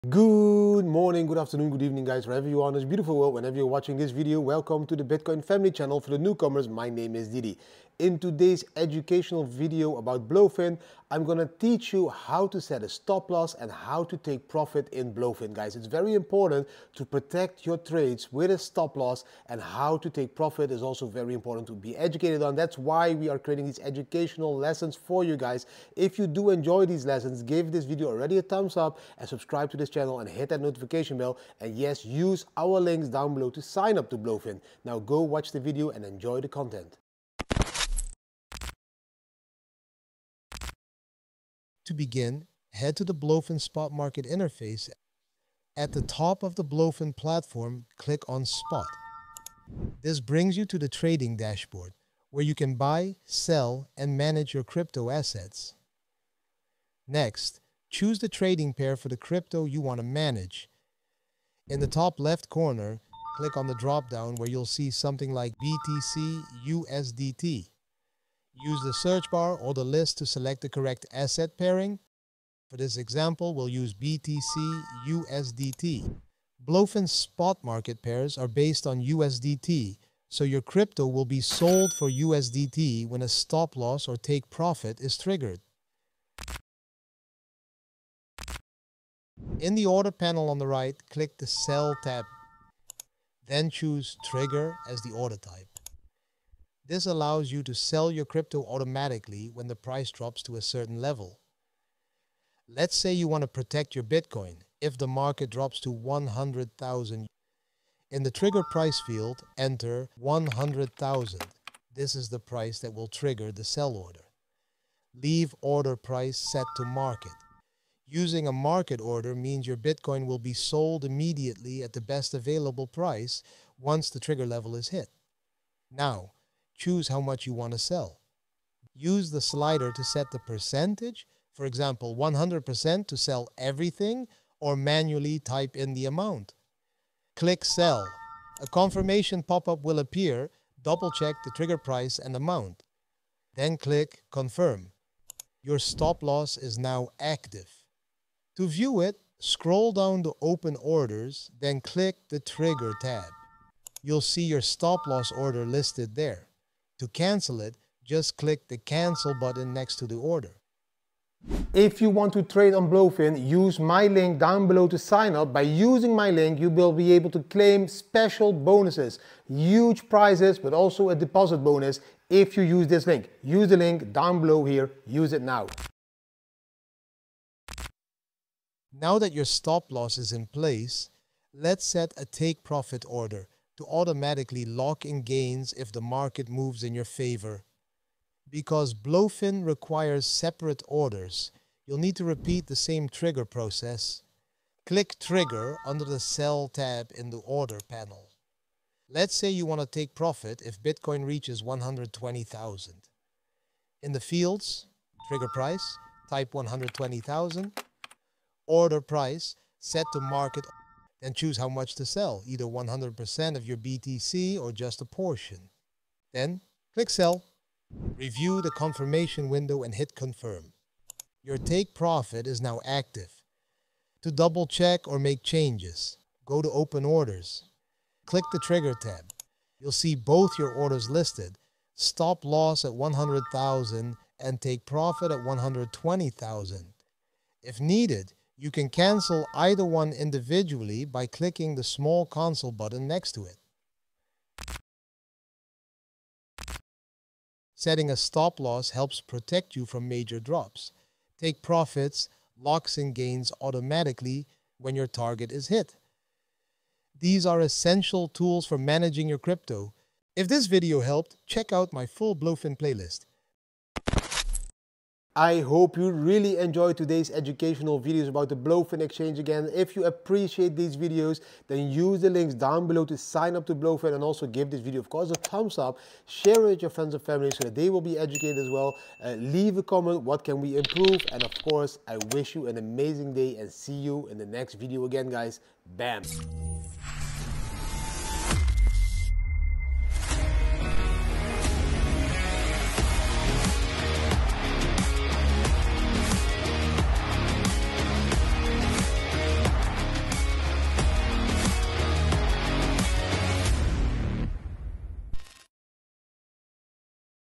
HOW TO SET A STOP LOSS AND TAKE PROFIT ON BLOFIN (NO KYC)
00:00 INTRO
00:39 SPOT MARKET INTERFACE
01:27 SETTING A STOP LOSS
02:56 SETTING A TAKE PROFIT
04:05 FINAL NOTE
04:24 OUTRO

🟠NOSTR Public key : npub1vmwjykc3rpmtpmrrlwrf4u8lfxdnnf0uj083fxtqm5gk2kgaee4qttdwtf